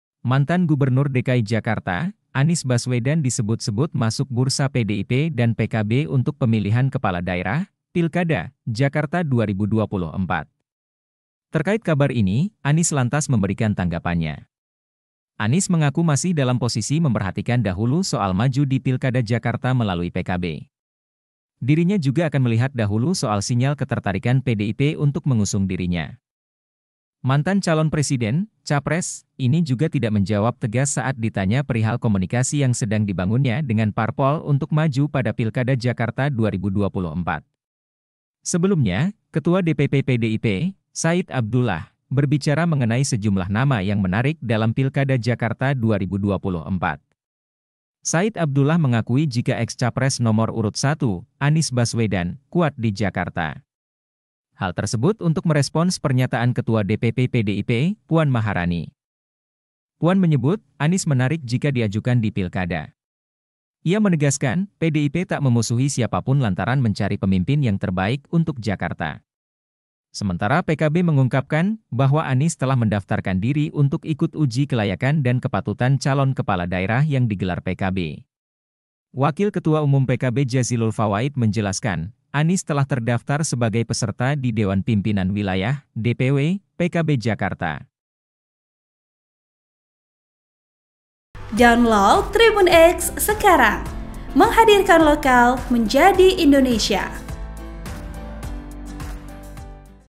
Tanggapan Anies Baswedan Dilirik PDI-P dan PKB untuk Pilkada Jakarta
Editor Video: Rizky Puji

TRIBUNJATIM.COM - Mantan Gubernur DKI Jakarta, Anies Baswedan disebut-sebut masuk bursa PDI-P dan PKB untuk Pemilihan Kepala Daerah (Pilkada) Jakarta 2024.

Terkait kabar ini, Anies lantas memberikan tanggapannya.

Anies mengaku masih dalam posisi memperhatikan dahulu soal maju di Pilkada Jakarta melalui PKB.

Dirinya juga akan melihat dahulu soal sinyal ketertarikan PDI-P untuk mengusung dirinya.

“Ah, kita lihat nanti, ngalir aja, ngalir aja,” kata Anies.

Mantan calon presiden (capres) ini juga tidak menjawab tegas saat ditanya perihal komunikasi yang sedang dibangunnya dengan parpol untuk maju pada Pilkada Jakarta 2024.

“Nanti dong, nanti kita lihat. Pokoknya saya masih lihat dulu sekarang,” kata Anies saat dikonfirmasi soal komunikasi dengan parpol.

Sebelumnya, Ketua DPP PDIP, Said Abdullah, berbicara mengenai sejumlah nama yang menarik dalam Pilkada Jakarta 2024.

Said Abdullah mengakui jika eks capres nomor urut 1, Anies Baswedan, kuat di Jakarta.

Hal tersebut untuk merespons pernyataan Ketua DPP PDIP, Puan Maharani.

Puan menyebut, Anies menarik jika diajukan di Pilkada.

"Fakta bahwa Anies kuat kita lihat Pilpres kemarin di Jakarta. Kalau Mbak Puan menyampaikan itu Pak Anies menarik, pasti menarik." ujarnya.

Ia menegaskan, PDIP tak memusuhi siapapun lantaran mencari pemimpin yang terbaik untuk Jakarta.

Sementara PKB mengungkapkan, bahwa Anies telah mendaftarkan diri untuk ikut uji kelayakan dan kepatutan calon kepala daerah yang digelar PKB.

Wakil Ketua Umum PKB Jazilul Fawaid menjelaskan, Anies telah terdaftar sebagai peserta di Dewan Pimpinan Wilayah (DPW) PKB Jakarta.